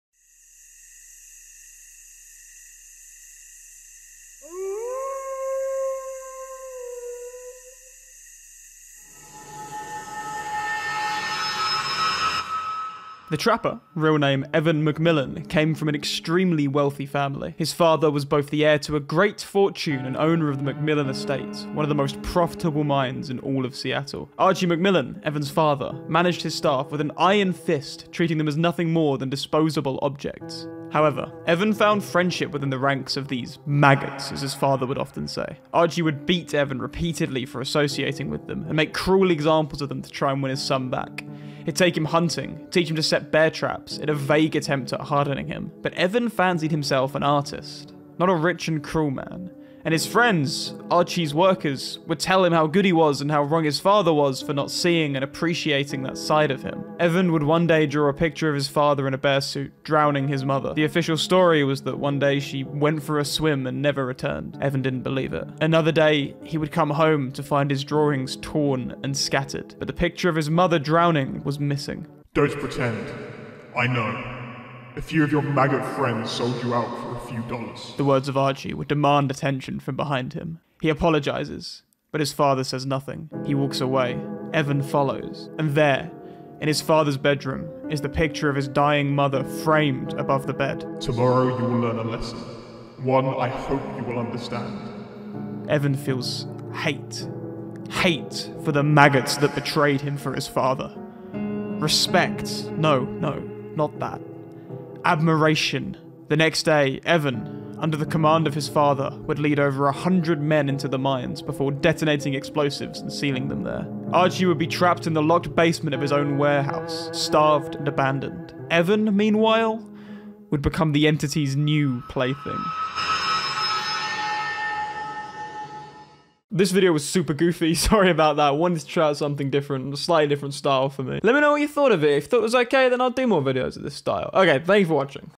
Quick Lore: The Trapper
In this video we take a brief look at the Trapper's Lore and what made him who he is in-game. This is me testing out a brand new style so if you liked it please let me know, and if you didn't then I, uh, guess let me know that too.